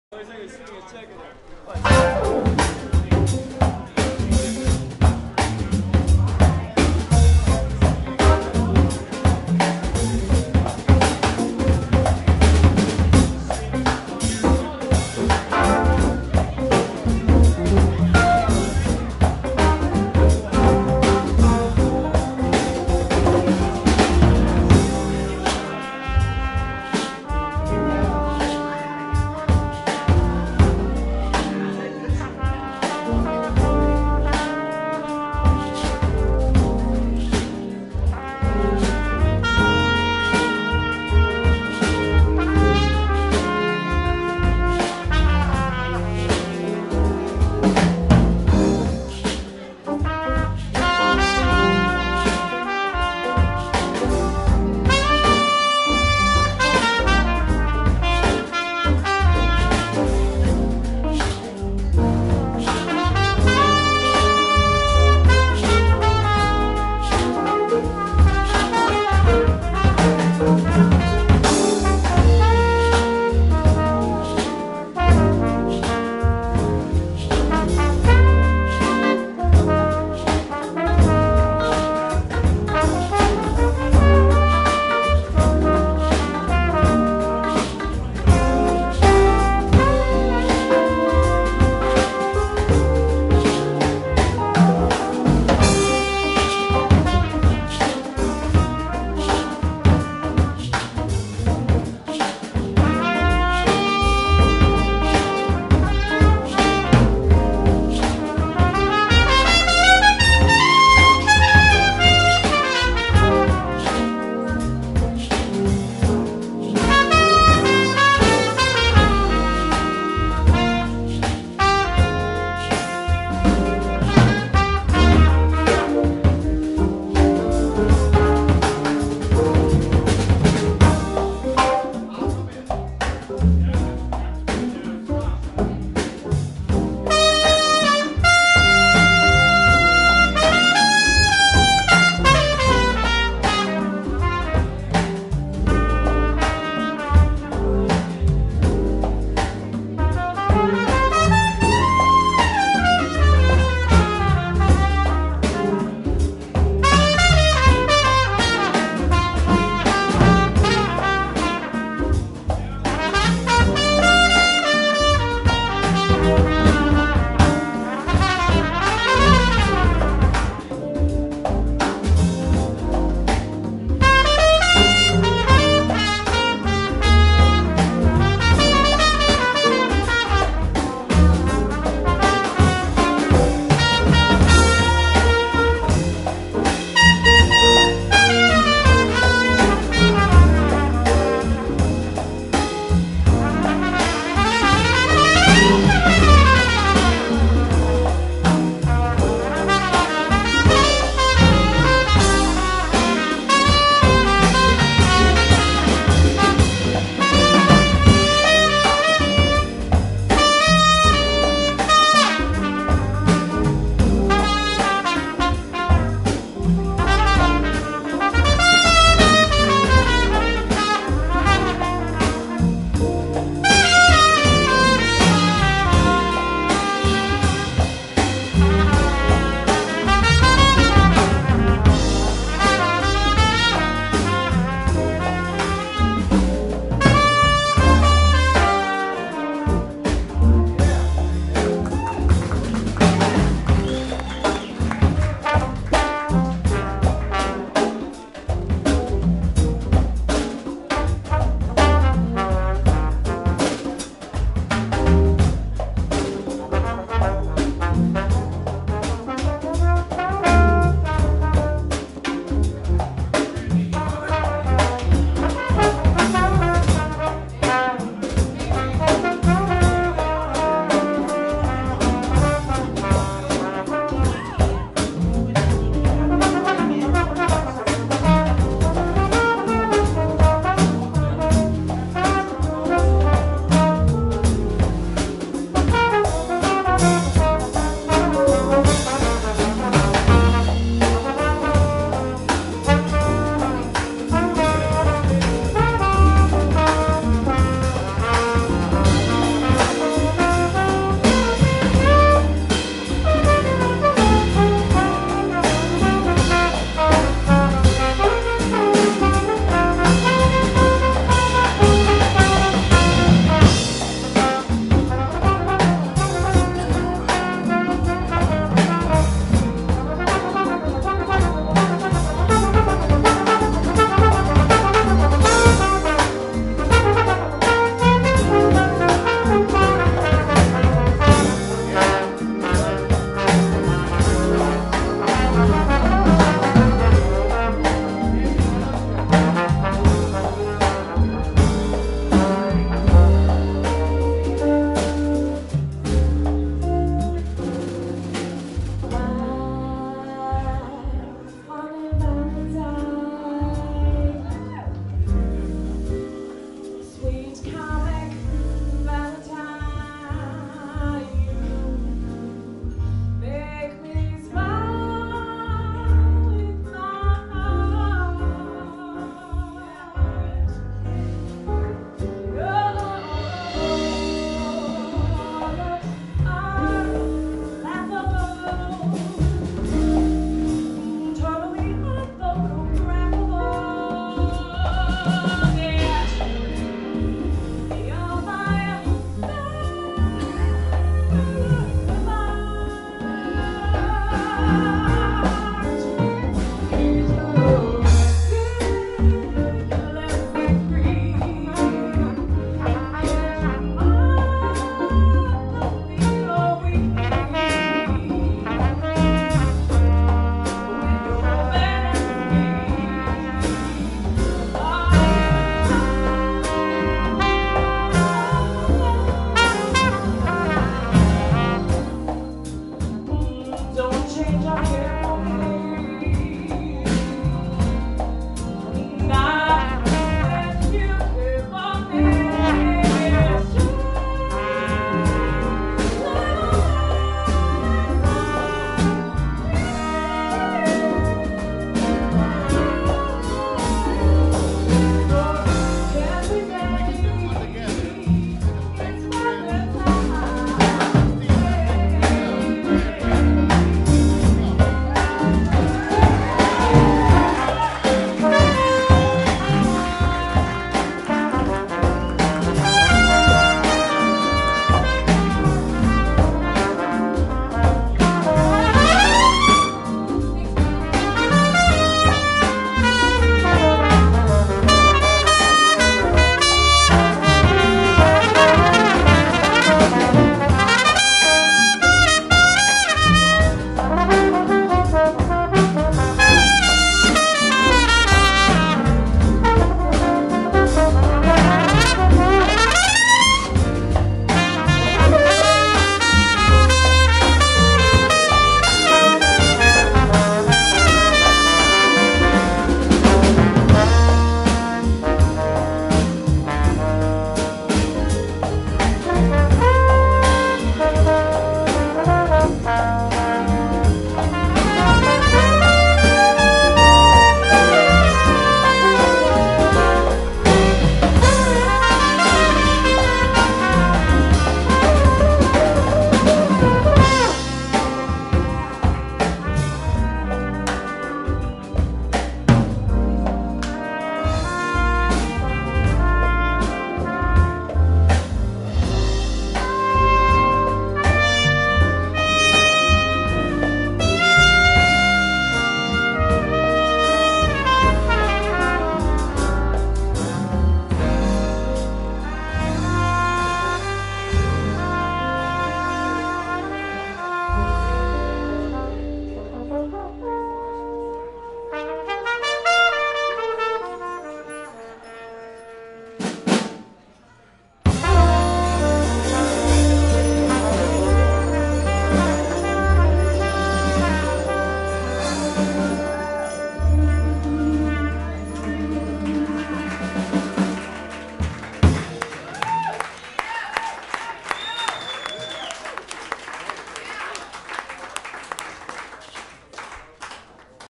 Speak EZ 11.08.15 Tune 2
Carlos Melendez (guitar), Mark Kahny (piano), Rob Mulligan (latin percussion), Eddie Eicher (drums), Dan Jacobs (trumpet), Jim Lutz (trombone)